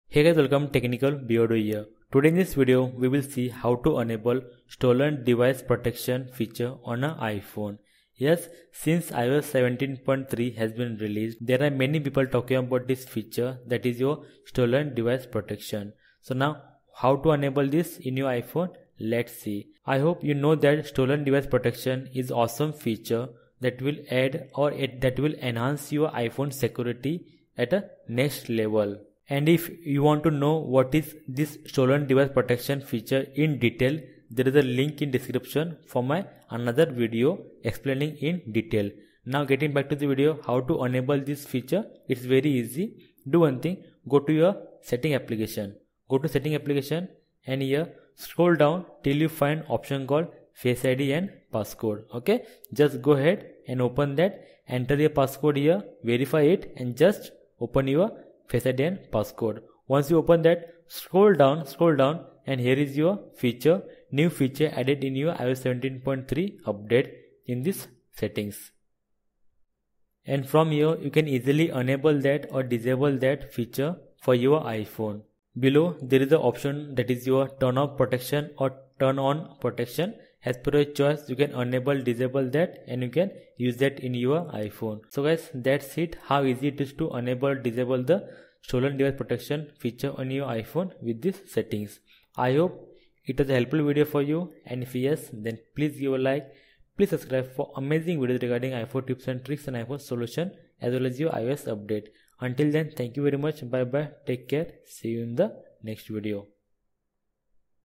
How to Enable Stolen Device Protection feature on iPhone
Stolen device protection feature is one of most useful feature recently added by Apple. In this video I have shown how you can enable Stolen device protection feature on iPhone. iOS 17.3 added this feature where it adds extra security layer for your iPhone.